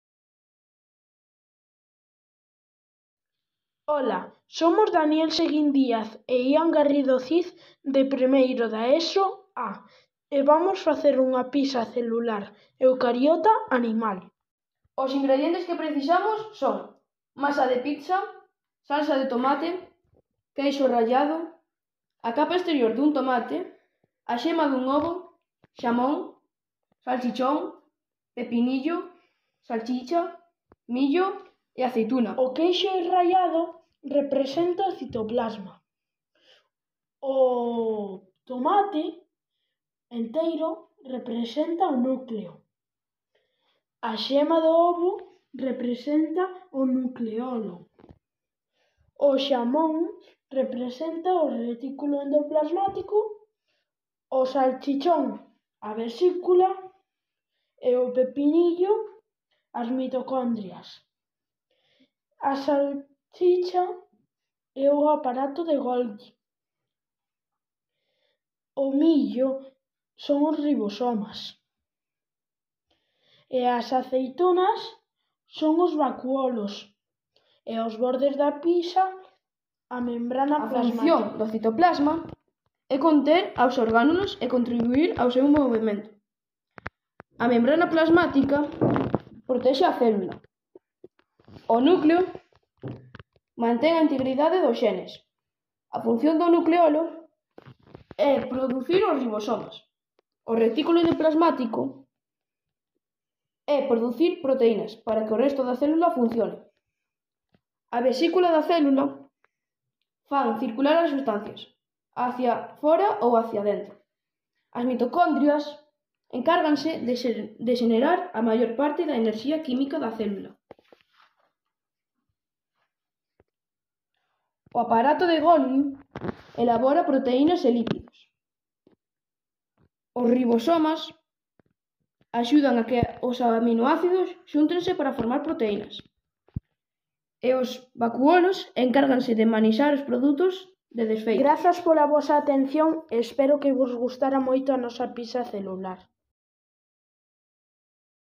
A pizza célula animal de Ian e Daniel
Ian Garrido e Daniel Seguín, alumnos de 1º ESO do IES de Allariz, presentan neste vídeo o seu modelo celular eucariota animal.